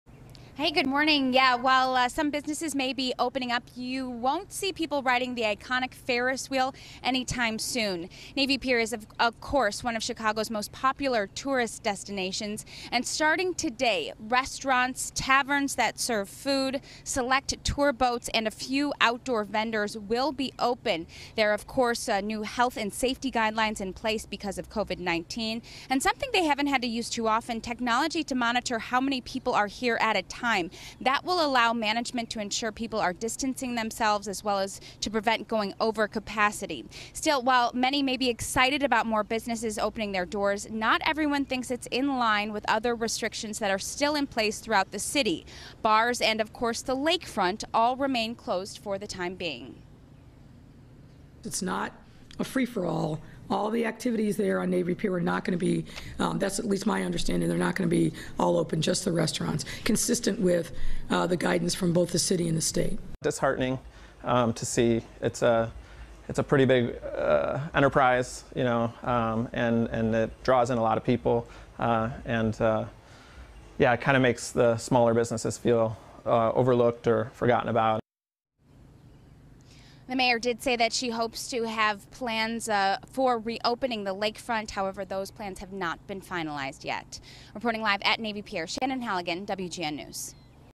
Navy Pier partially reopens after shutdown due to pandemic
Chicago's Navy Pier will partially reopen Wednesday after being shutdown due to the coronavirus pandemic